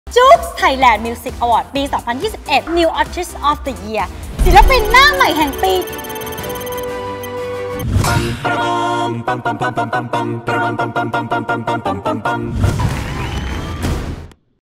ลุ้นผลรางวัล NEW ARTIST OF THE THE YEAR ได้แล้วตอนนี้ที่ JOOX BUZZ
ใครจะคว้ารางวัล NEW ARTIST OF THE THE YEAR ในงาน JOOX Thailand Music Awards 2021 ไปครอง ลุ้นผลพร้อมกันตอนนี้ที่